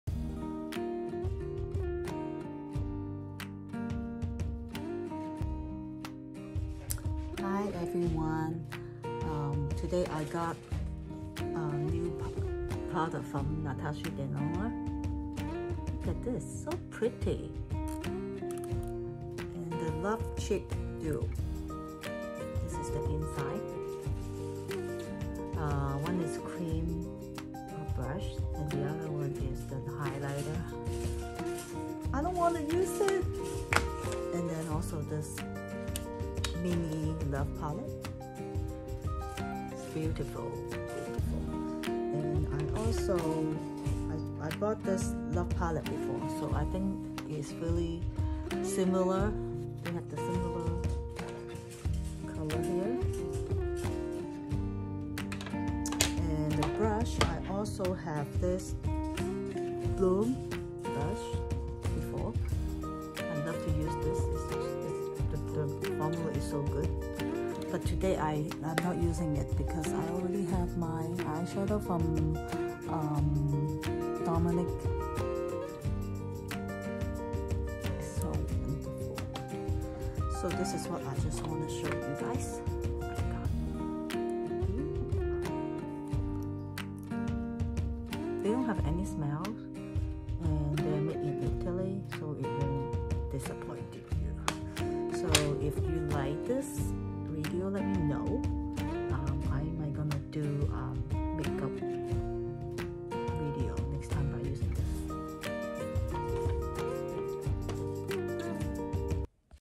Natasha Denona Mini Palette/ Love Cheek Duo
Unboxing my new ND love palette and cheek duo, they are beautiful and gorgeous.  I will start using it by next video and with love palette from ND also.

Follow Instagram: princesszeemode

TopCasBack - Get Cash Back when you shop online!!